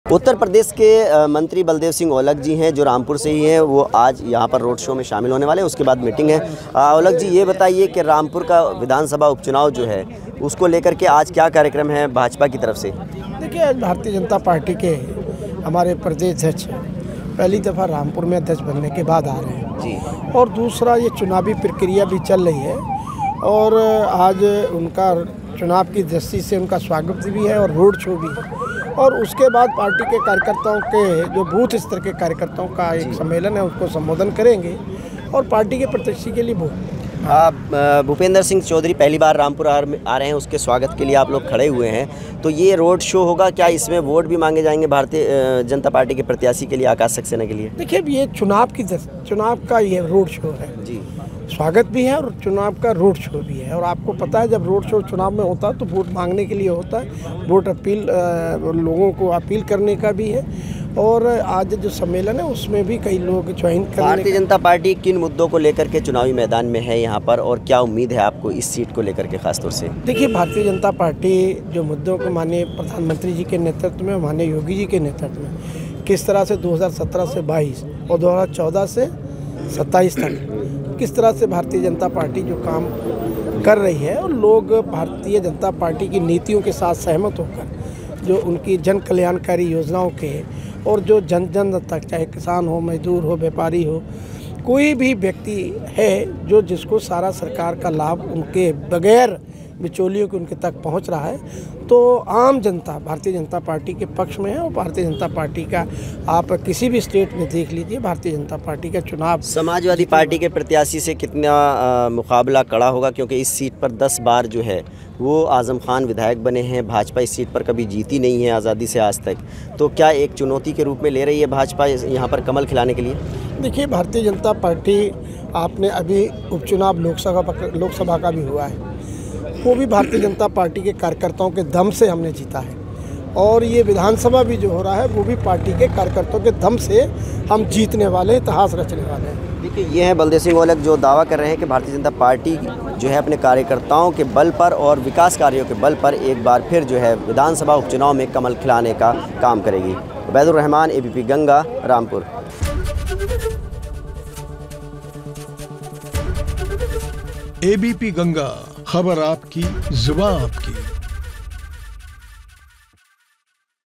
क्या Rampur जीत पाएगी BJP ?, इस पर क्या बोले मंत्री बलदेव सिंह औलख
यूपी के मंत्री बलदेव सिंह औलख ने रामपुर में आकाश सक्सेना के लिए रोड शो में शामिल हुए.